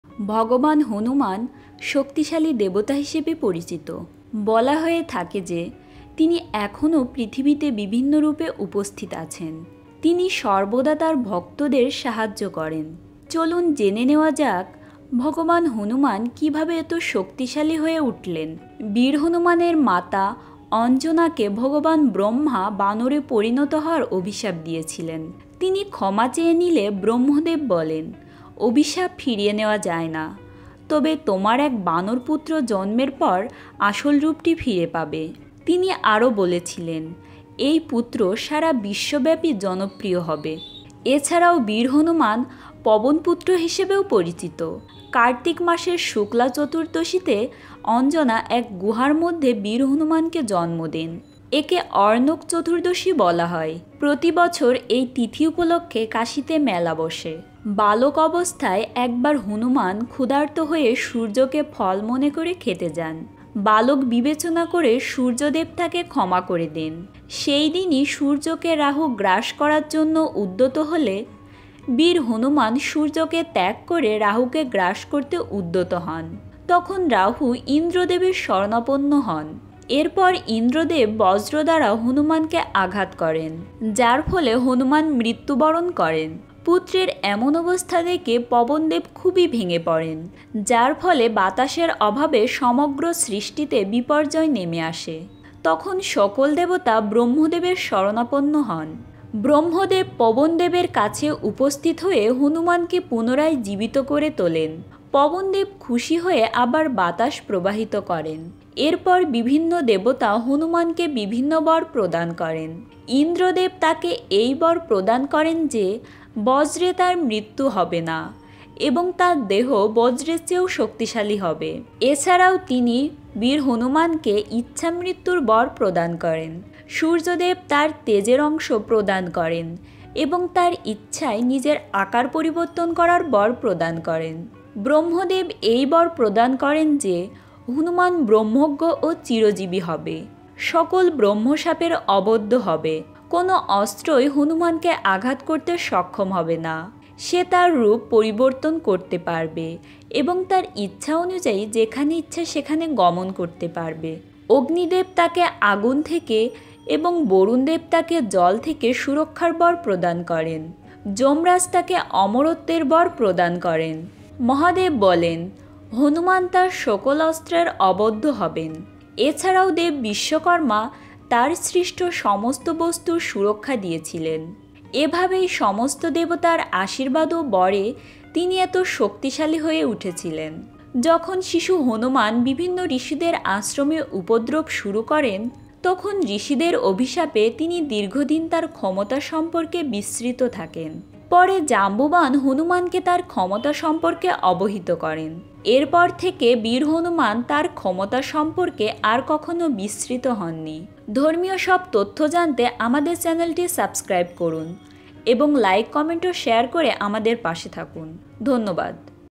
হনুমান এতো শক্তিশালী হলো কী করে । How Hanuman became so powerful
ভগবান হনুমান শক্তিশালী দেবতা হিসেবে পরিচিত। বলা হয়ে থাকে যে তিনি এখনো পৃথিবীতে বিভিন্ন রূপে উপস্থিত আছেন। তিনি সর্বদা তার ভক্তদের সাহায্য করেন। চলুন জেনে নেওয়া যাক ভগবান হনুমান কীভাবে এতো শক্তিশালী হয়ে উঠলেন?